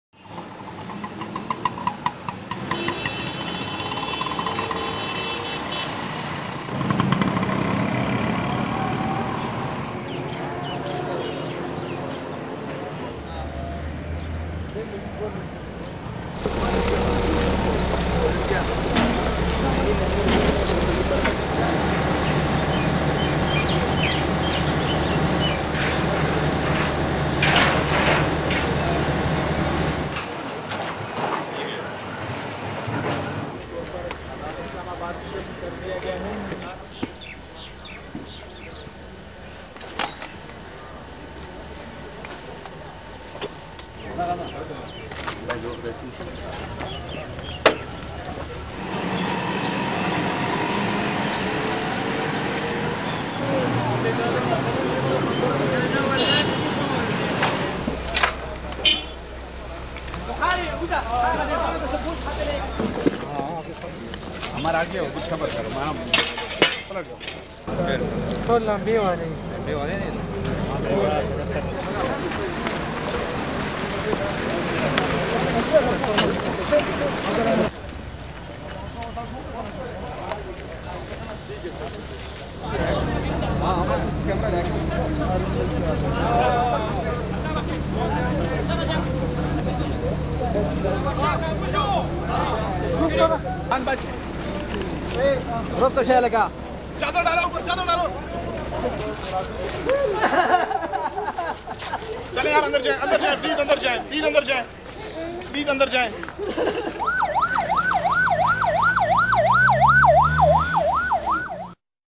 Peshawar Pearl Hotel clean up
Authorities cleaning up Pearl Hotel in Peshawar, Pakistan